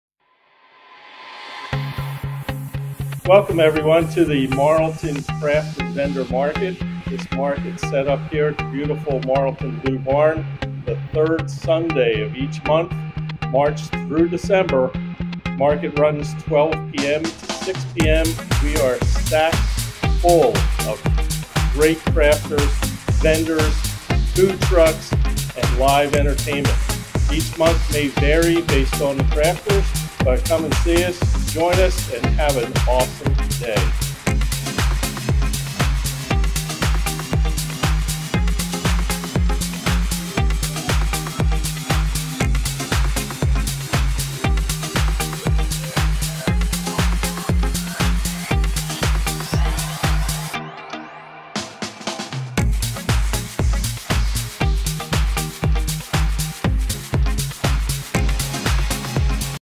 The Marlton Craft & Vendor Fair - Evesham Economic Council Spotlight
The Evesham Economic Advisory Council spotlights The Marlton Craft & Vendor Market!